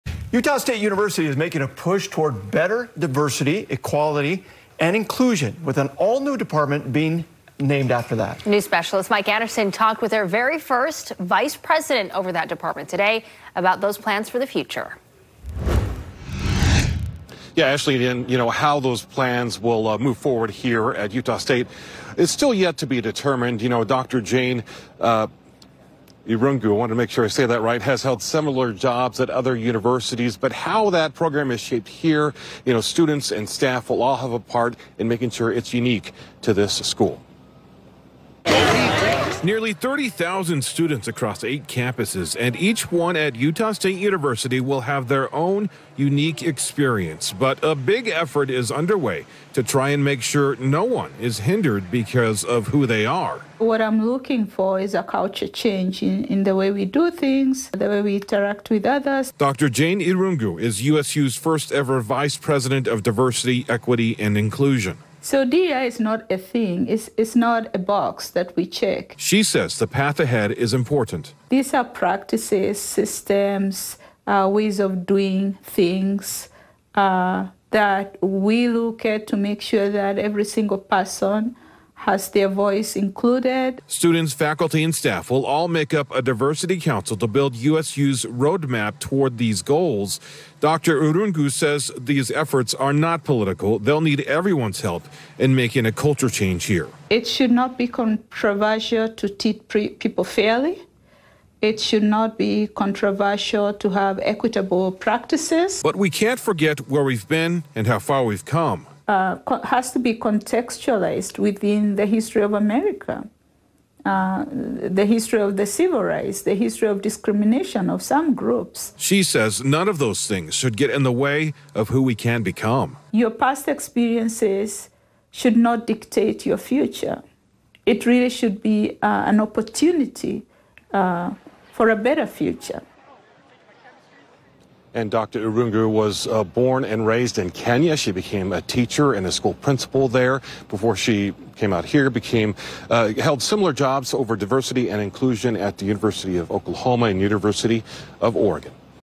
Utah State University forms new division, focusing on diversity, equity and inclusion
Utah State University is pushing toward better diversity, equity, and inclusion with an all-new department bearing that name.